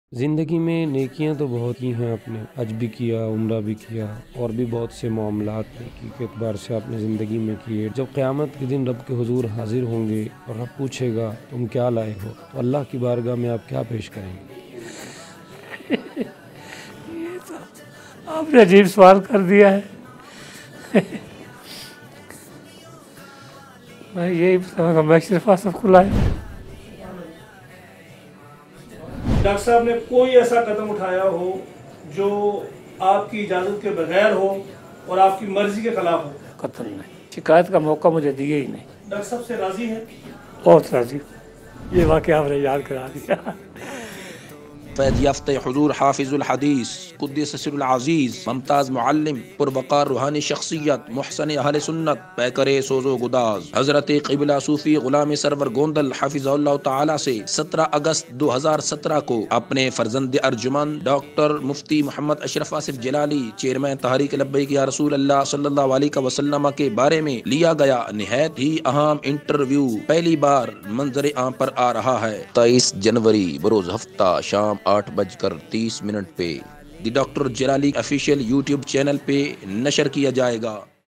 Sufi Ghulam Sarwar Gondal | Father Of Dr Ashraf Asif Jalali | Interview Promo | About Life
Father Of Dr Ashraf Asif Jalali Hazrat Qibla Sufi Ghulam Sarwer Gondal | Interview Promo About Dr Ashraf Asif Jalali |